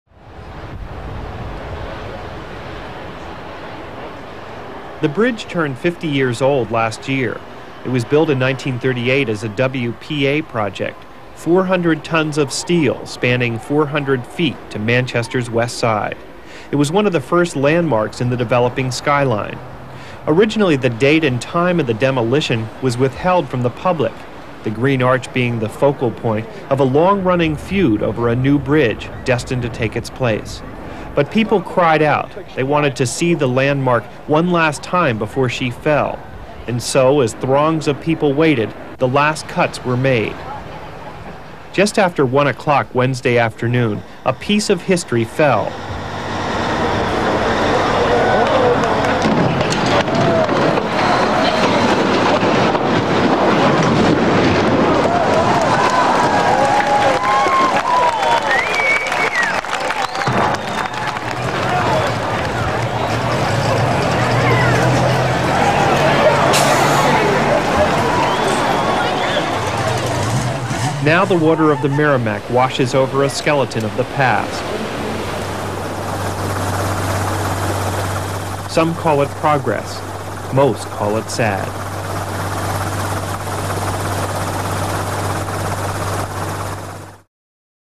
Notre Dame Bridge demolition Machester NH 6 September1989
It was a warm late summer Wednesday in 1989 (update - it was September 6), when four hundred tons of steel were pulled from the Manchester (NH) skyline to make room for bigger interchanges to accommodate the increasing demands of growing automotive population.

Normally when I covered quick-hit stories for New Hampshire Public Television's weekly NH Journal, I let the people in the pieces tell the story. Time did not allow that approach for this one: beside the fact that most bridges don't, in fact, speak, word only came down on a Friday that the bridge was coming down sometime the following week...no time to line up the usual suspects for a brief history and commentary on the event. Indeed, the program for that week didn't have a lot of room for a short feature. In fact, I was only able to squeeze a quick jaunt down to Manchvegas following a shoot in Concord that morning, with a Seacoast event shortly after the demolition was scheduled.

I did have time though, to get hold of Dean Kamen (yes, the Segway guy) beforehand to line up a ride over, as he called it, "the scene of the crime" when all was said and done. Ten minutes after the steel was soaked, I was climbing into his Enstrom on the roof of the building next door. My news director asked me to write something, so I did, and, as I was not on-air talent, he voiced it for me, without changing any of the words I had typed out as a quick draft that afternoon.

-- 2019 Update: It occurred to me that it's been thirty years since this bridge came down, and while looking for web-enshrined nostalgia I recently discovered a site with a great sequence of photos, in amazing black-and-white* by the Union Leader's George Naum, from the next roof downstream from mine.

* to borrow from Rhymin' Simon, everything looks better in black and white.